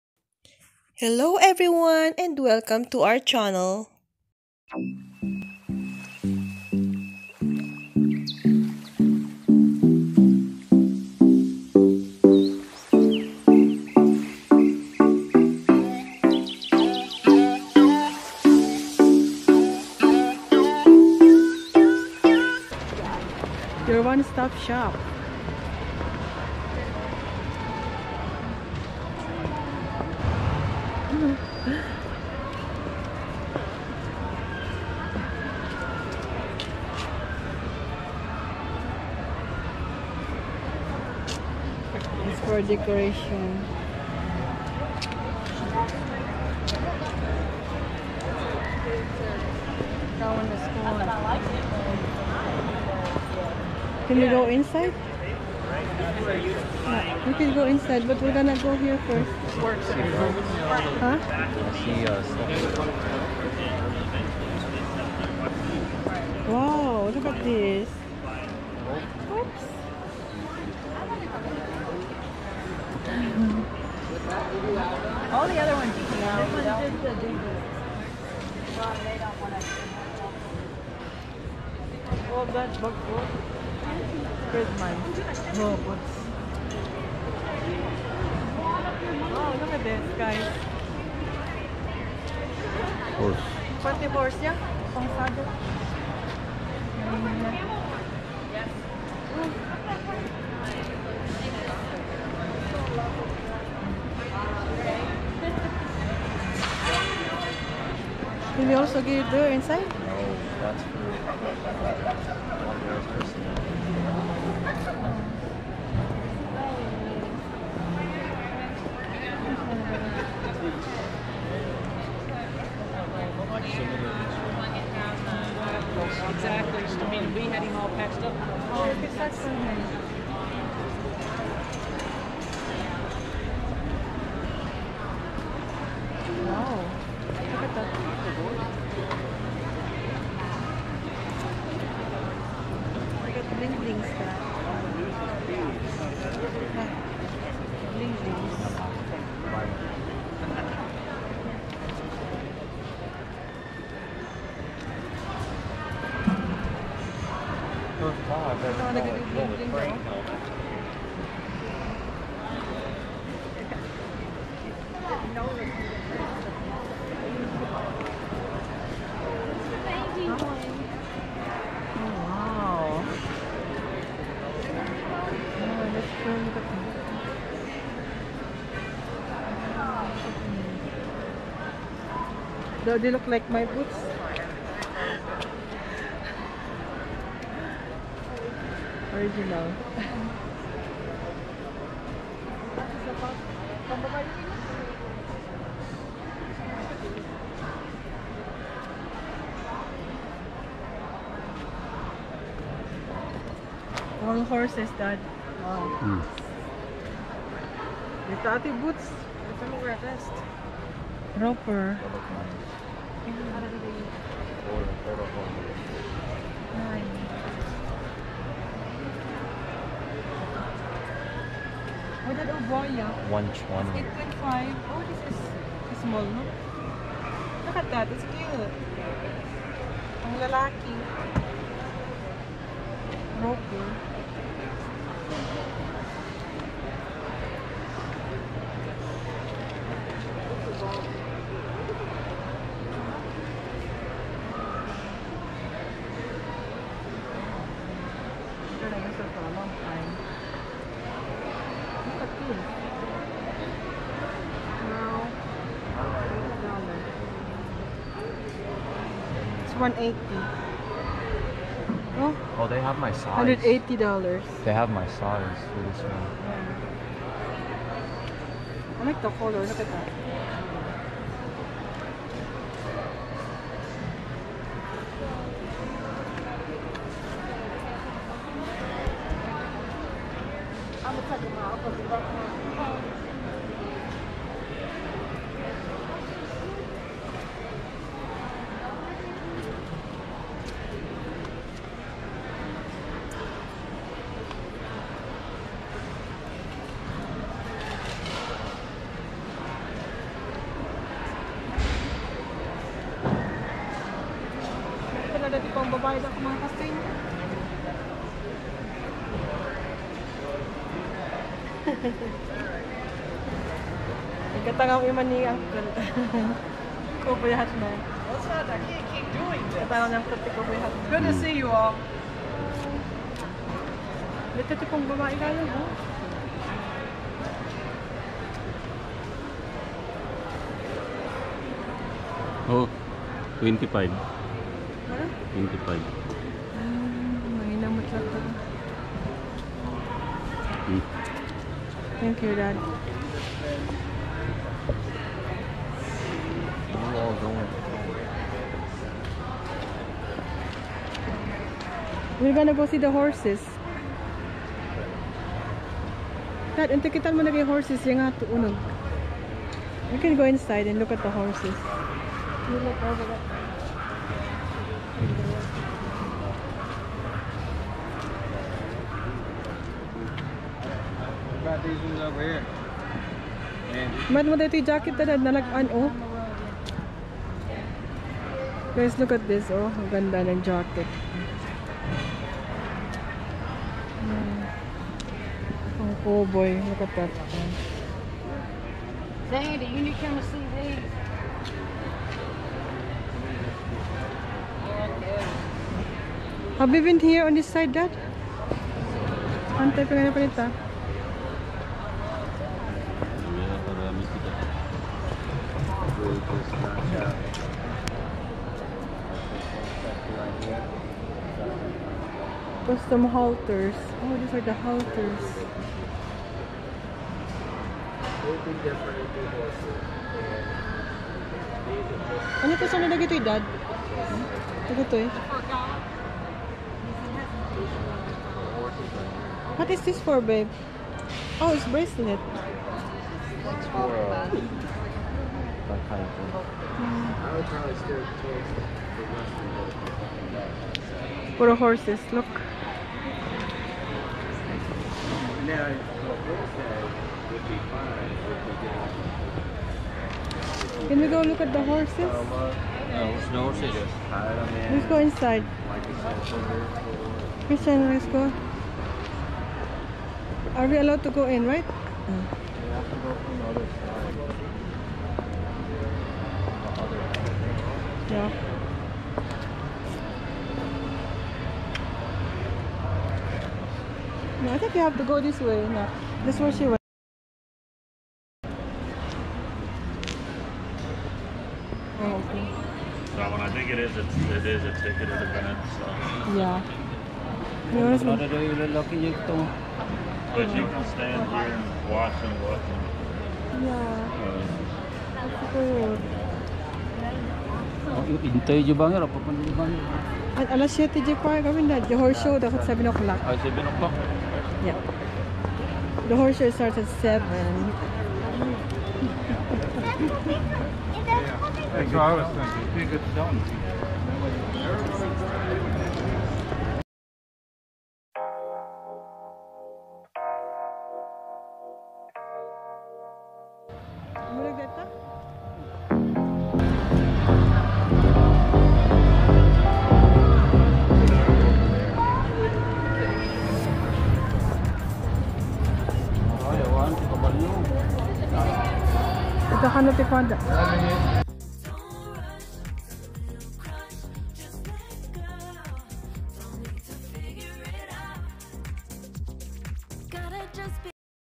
2021 Kentucky State Fair/ COWBOY BOOTS for Sale !filipinosinKentucky
Thankful to be able to go to the State Fair this year at the Kentucky Exposition Center.
So lovely to see some of the  areas and be able to get footage to share to everyone who can't go.
Watch this video to see some of the bazaars or shops we checked.
Hmmm. Saw some of our favorites.
Hope you like this video. More uploads coming to see more.
Many thanks again. Salamat Po.